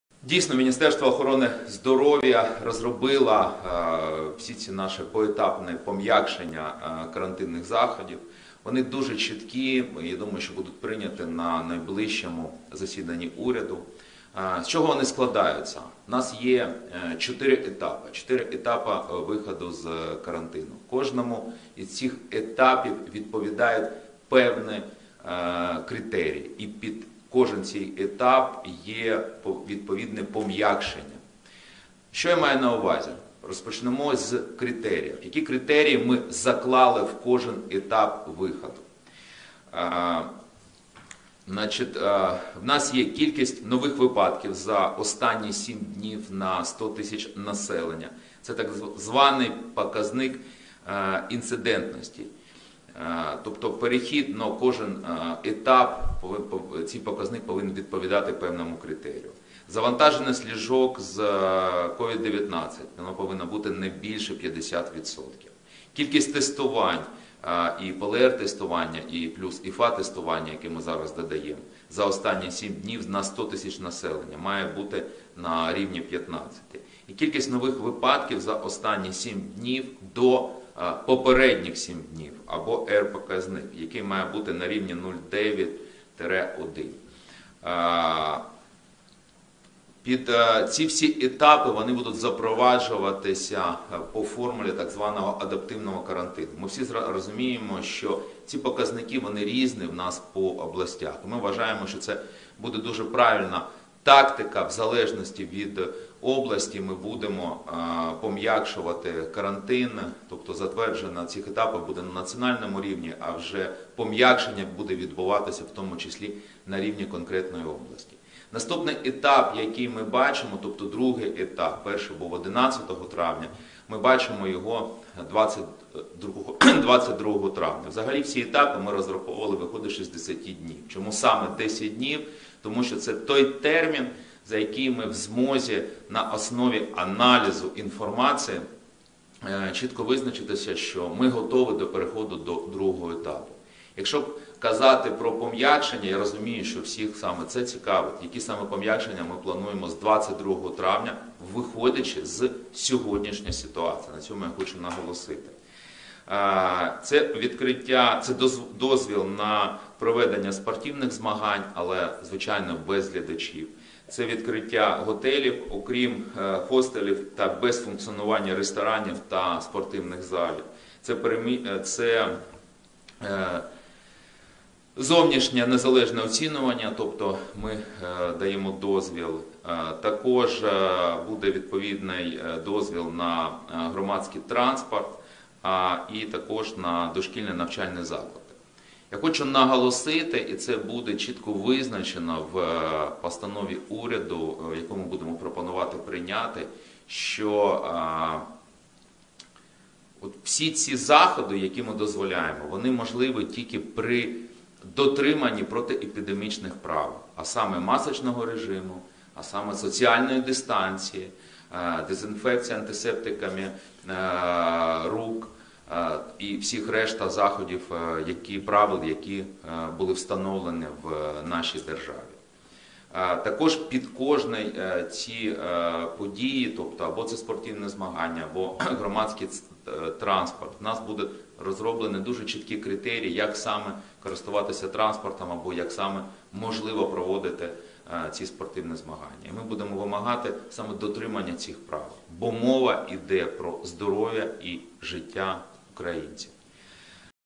Глава МОЗ о втором этапе послабления карантина в Украине | 1kr.ua
22 мая в Украине планируют ослабить карантин, объявленный из-за коронавируса. Планируется разрешить работу отелей и общественного транспорта. Какие еще ограничения планируют снять – смотрите в видео.